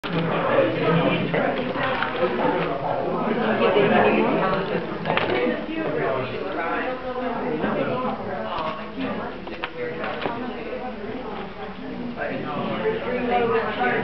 Flotsam at the 11-11-11 event
This is a quick fly-by of some of the inter-dimensional flotsam that was in evidence during the 11-11-11 event.